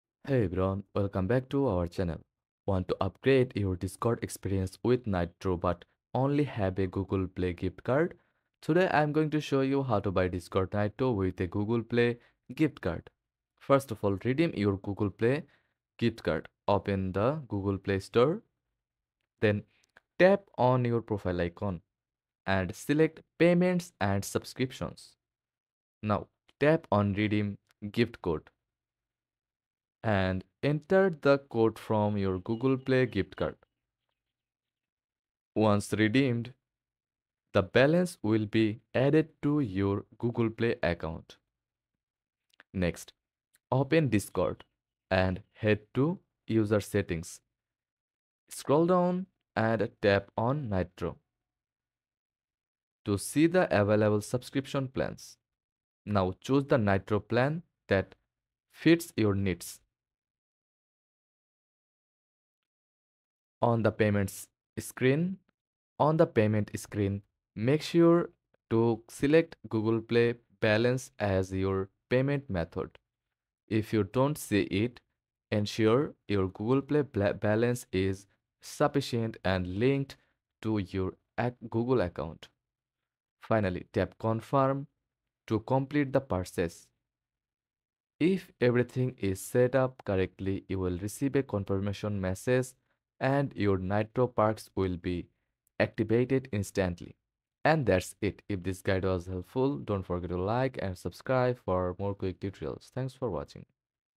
HOW TO BUY DISCORD NITRO WITH GOOGLE PLAY GIFT CARD - 2025
In this video, we’ll show you step-by-step how to buy Discord Nitro using a Google Play gift card. Learn how to redeem your Google Play balance and easily use it to purchase Discord Nitro to unlock all its premium features.